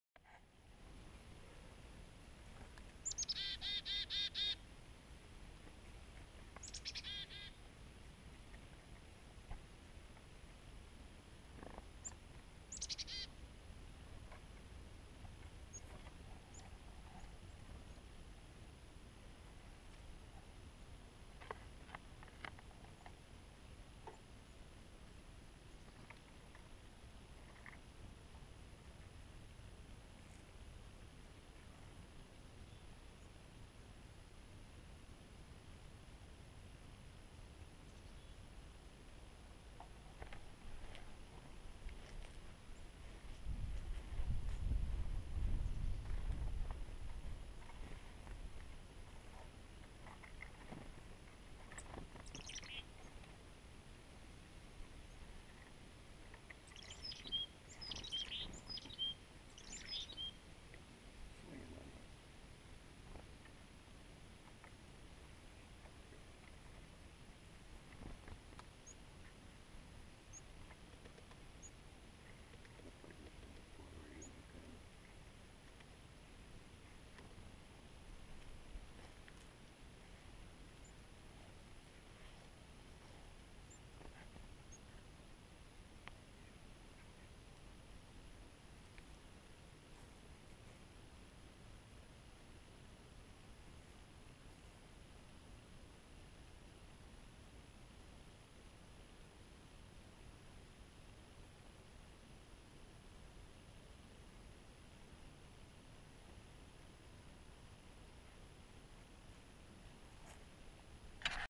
Chickadee birds in the feeder.
These birds are so cool. They will come eat sunflower seeds right out of your hand. They love people.